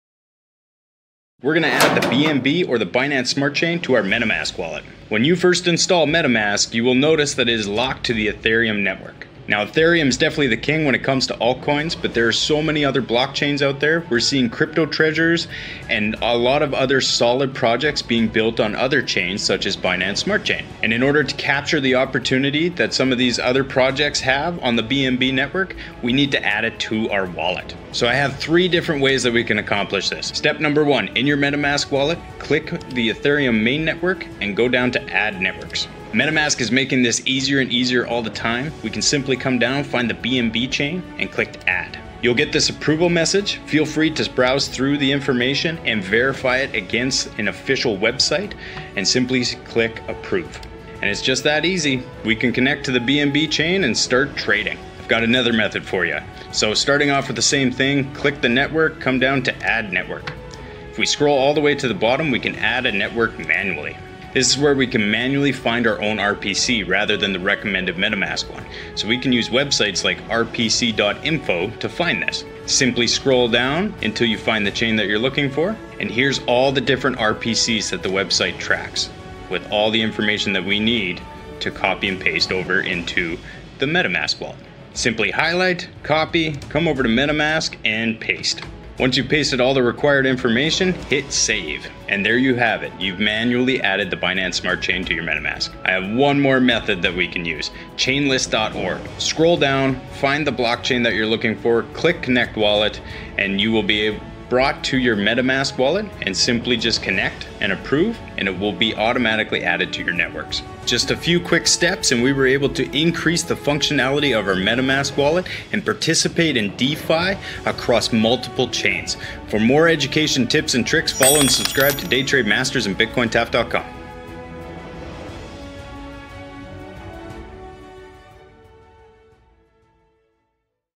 How to Add the Binance Smart Chain to MetaMask
Learn 3 Easy Ways to Increase the Functionality of your Wallet with this Quick Tutorial & Unlock DeFi Potential 💰